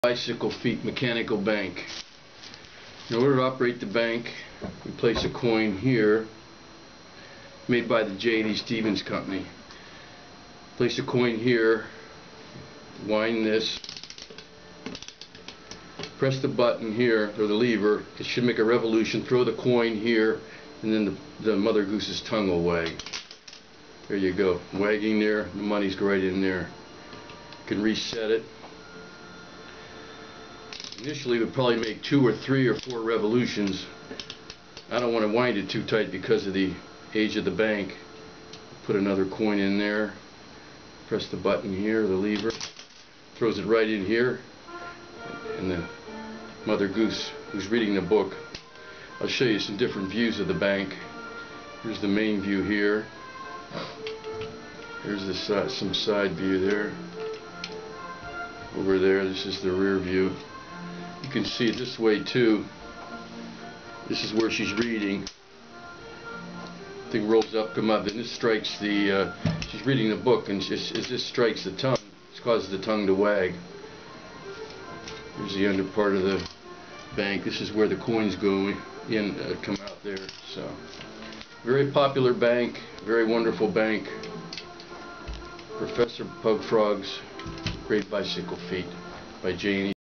"PROFESSOR PUG FROG'S GREAT BICYCLE FEAT" Original Antique Mechanical Bank Video
This is a brief descripion of the Professor Pug Frog's Great Bicycle Feat mechanical bank and shows how it operates. Video by Jack Allen on January 31, 2014.